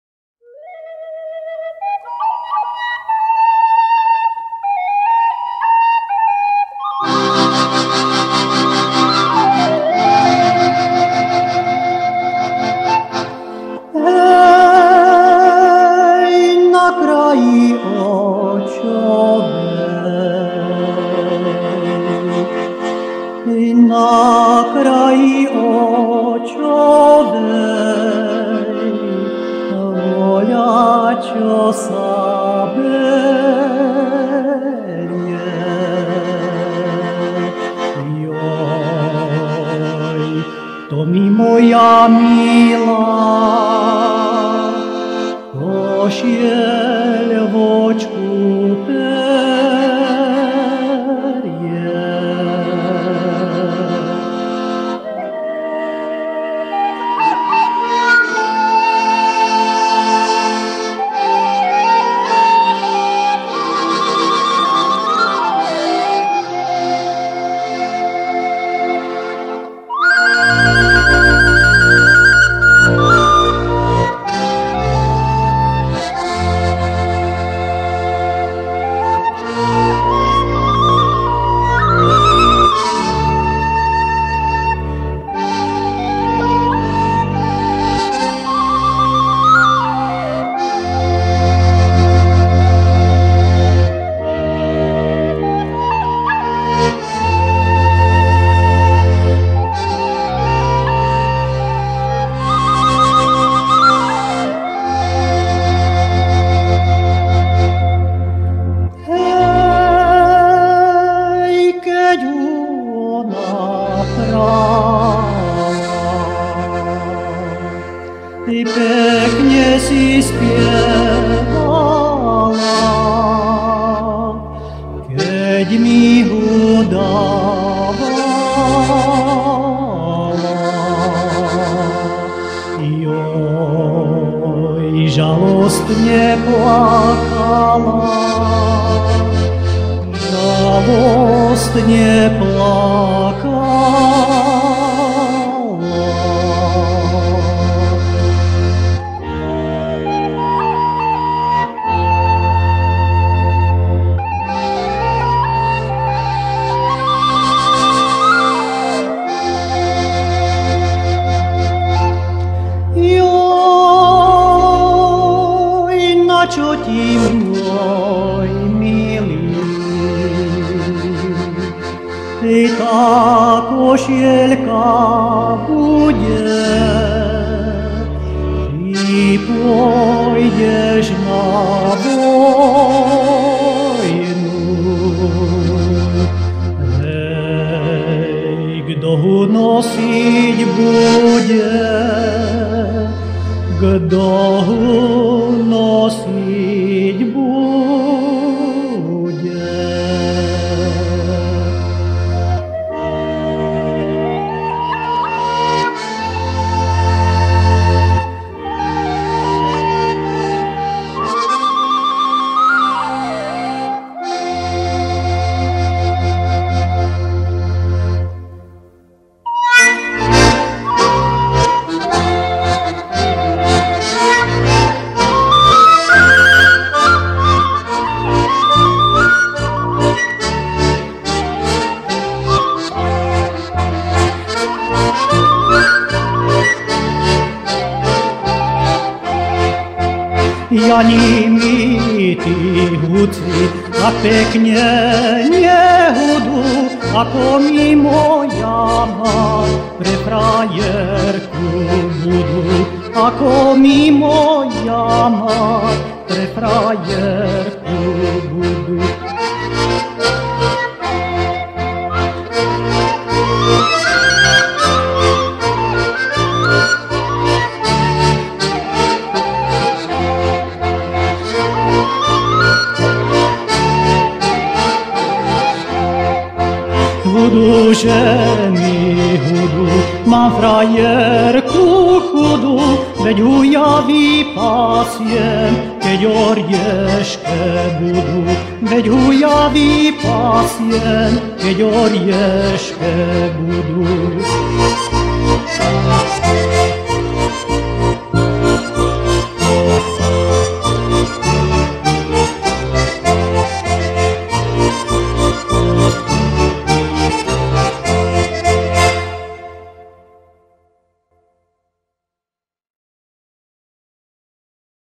Artikulárny kostol Leštiny
Fotoprezentácia z návštevy vzácnej pamätihodnosti na Orave s hudobným doprovodom Dušana Holíka